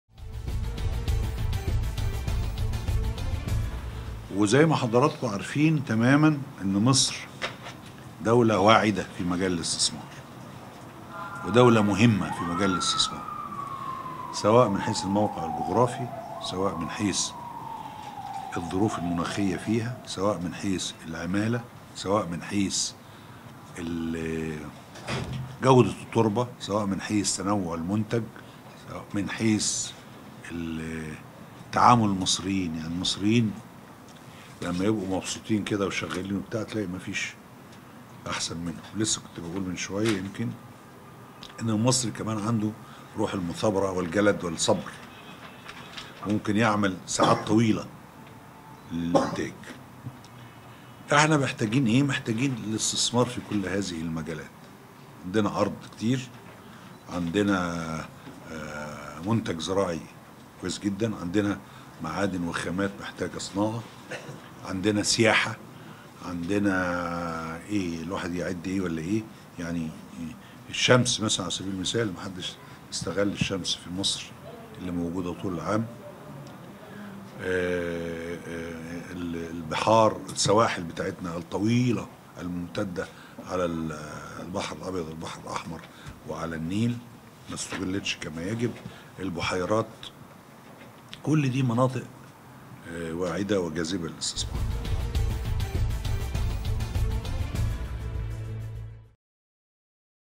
مصر العربية | تشريع مجلس الدولة : مصر دولة واعدة فى الاستثمار
قال المستشار أحمد أبو العزم، رئيس قسم التشريع بمجلس الدولة أن مصر دولة واعدة فى مجال الاستثمار سواء من حيث العمالة او الظروف المناخية او المناخ الاستثمارى